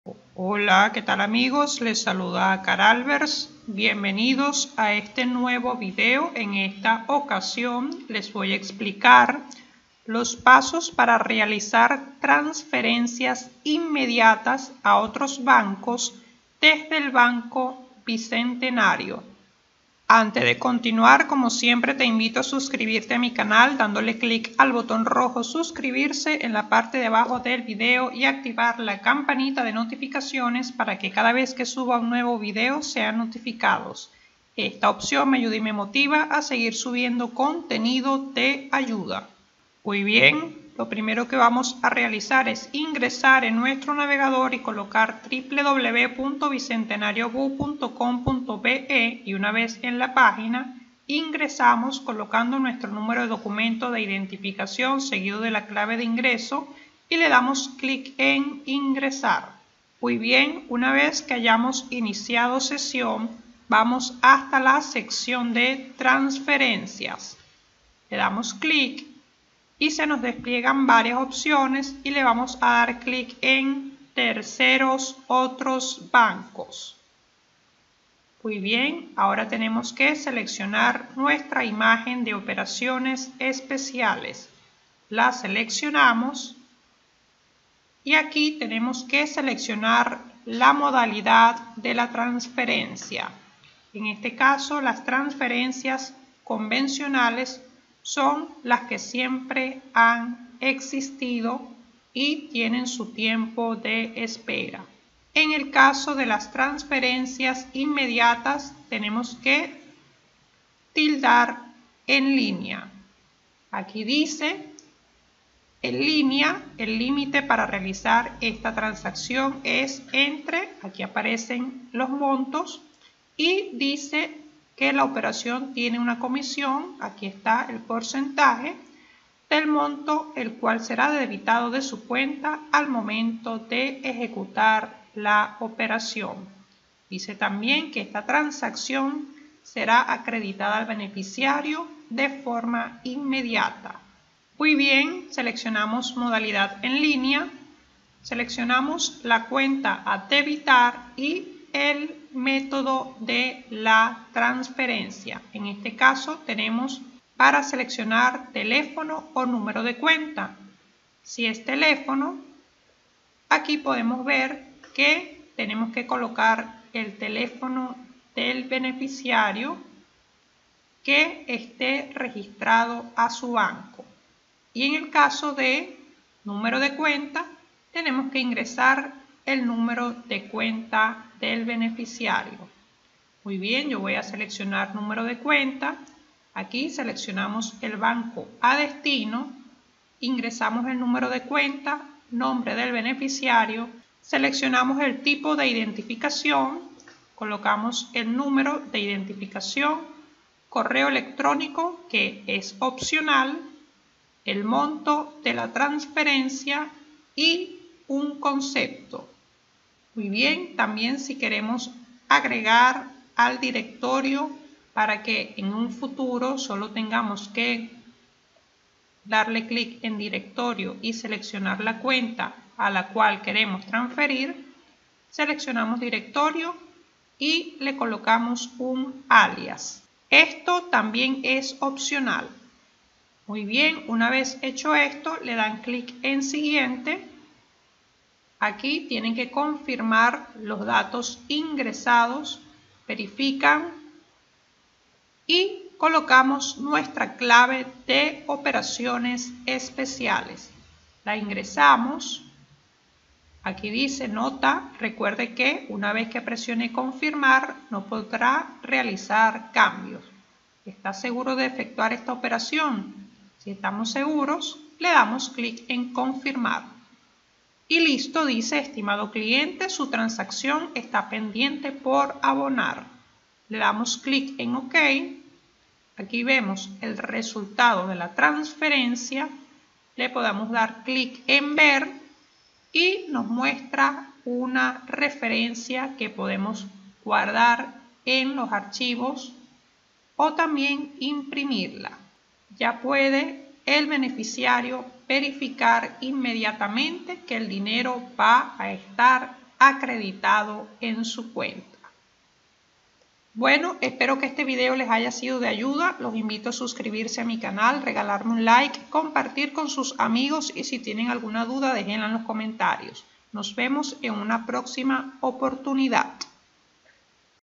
Cómo Realizar Transferencias Inmediatas a Otros Bancos. Banco Bicentenario. 2022.
Business | Publicidad | Contacto

¡Saludos! En este video les explico los pasos para realizar Transferencias Inmediatas a Otros Bancos, desde el Banco Bicentenario a través de su banca en línea. Espero que les sea de ayuda.

Este video no incluye publicidad